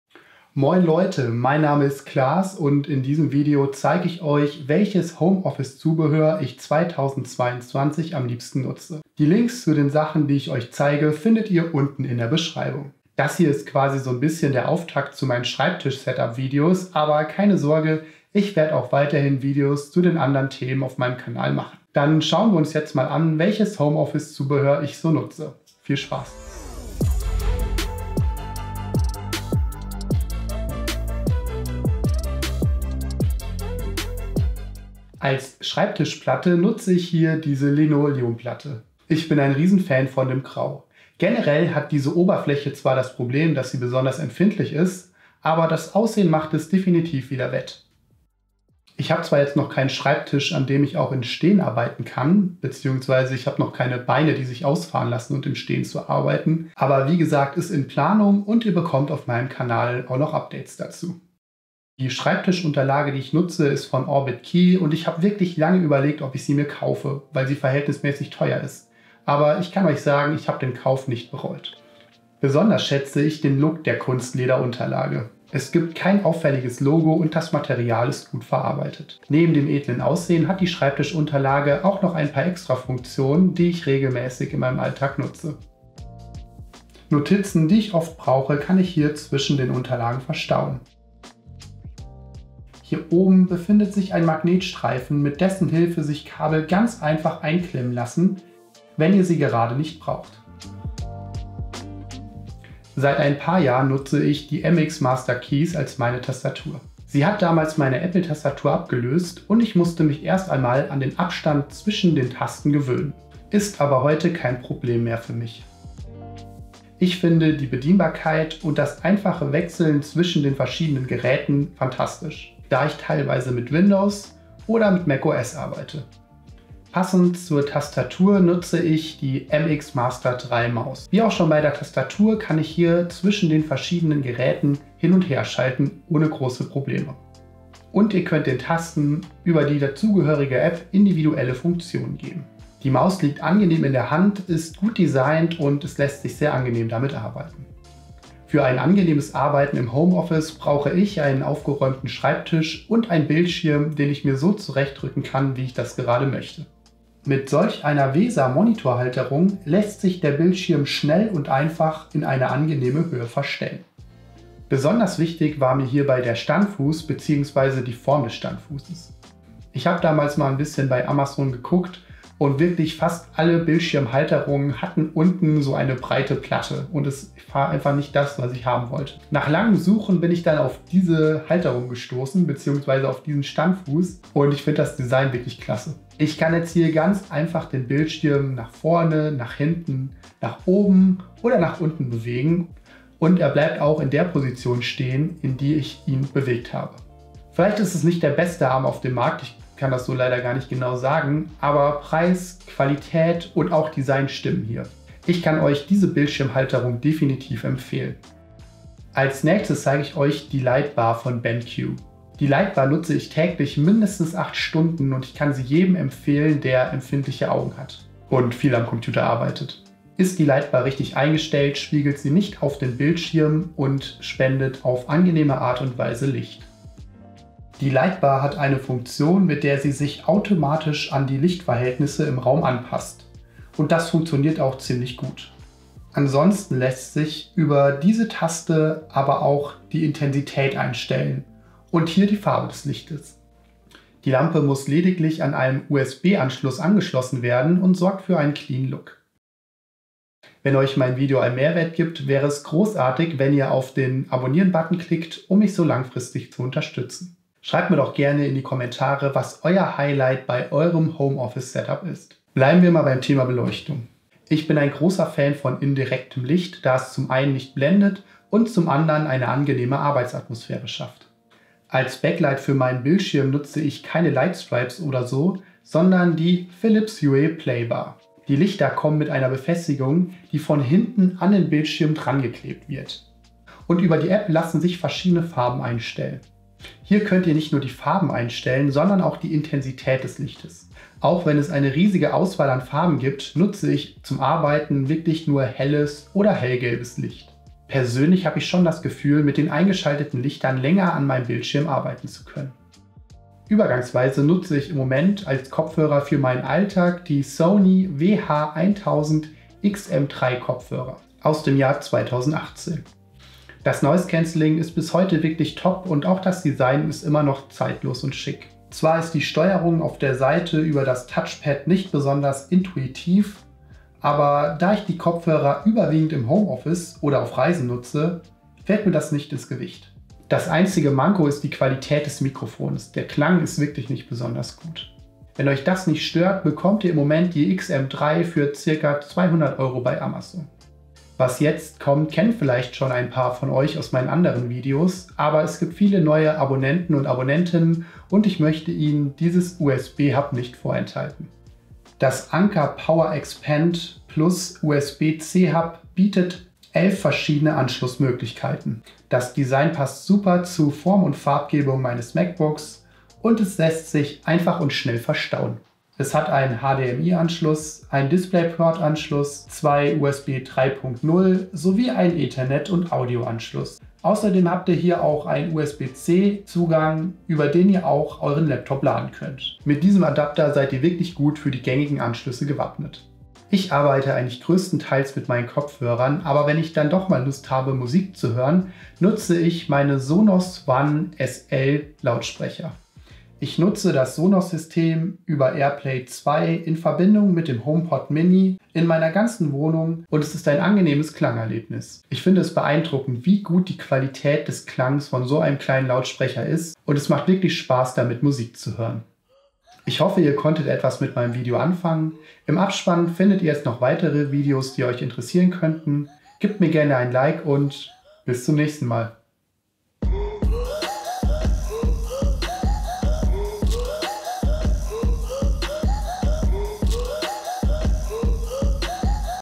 Top 10 Schreibtisch Setup Zubehör zum Homeoffice einrichten (Deutsch)
Ich zeige euch mein Homeoffice Schreibtisch Setup 2022 mit verschiedenen Produkten die mir den Alltag erleichtern und womit ihr euer Home Office Setup einrichten könnt. Vielen Dank fürs Anschauen!

Für euch entstehen keine Kosten oder Nachteile, wenn ihr auf den Link klickt. Kauf über diesen Link erwirtschaftet eine einstellig prozentuale Provision.

Inhalt:
0:00 Homeoffice einrichten: Mein Schreibtisch Setup
1:00 Schreibtischplatte und Schreibtischunterlage
1:42 Tastatur
2:05 Maus
2:36 Bildschirmhalterung
3:28 Lightbar / ScreenBar BenQ
4:09 Hintergrundbeleuchtung für Bildschirm
5:03 Kopfhörer
5:51 USB-Adapter
6:23 Lautsprecher